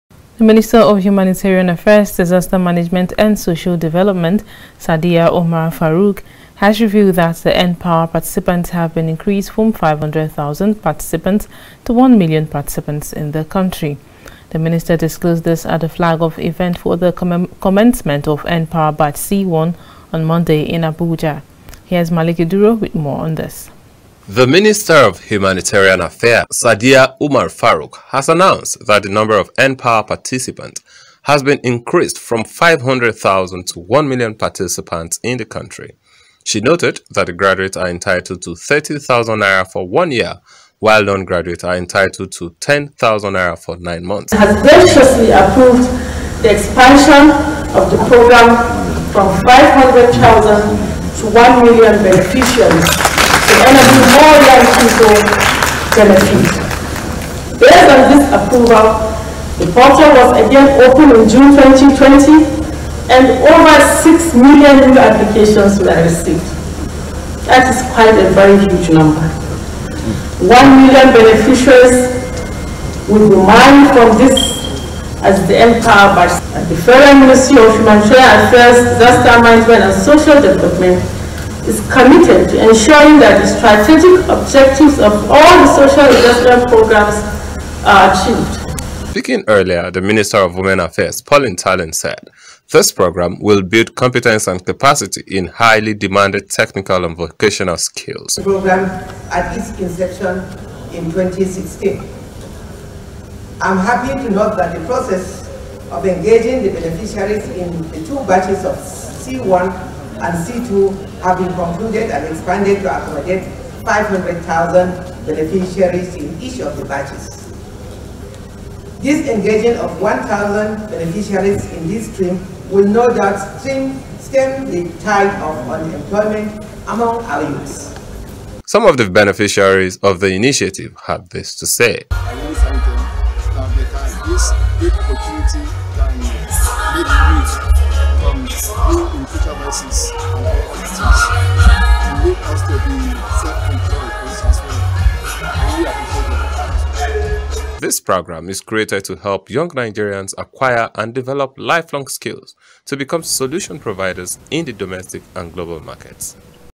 N POWER
FG Increases N-POWER Participants From 500,000 To 1 Million.